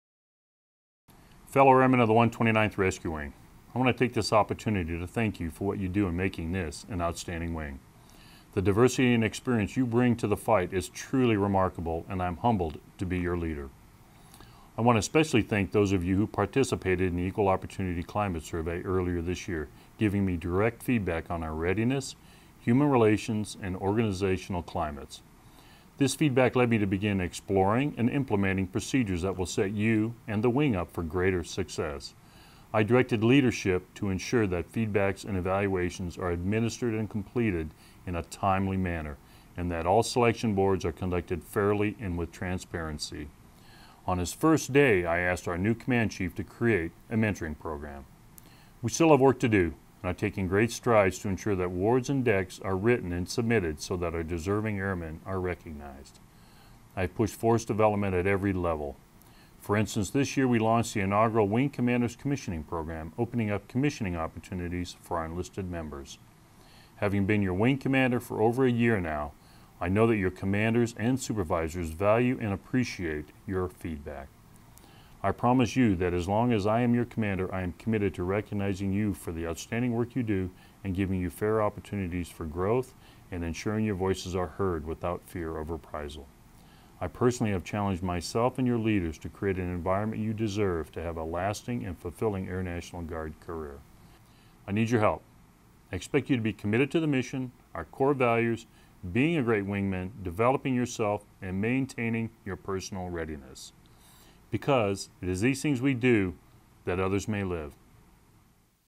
Words from the Boss
Colonel Gregory F. Jones, Commander, 129th Rescue Wing, shares some thoughts regarding the wing's organizational climate.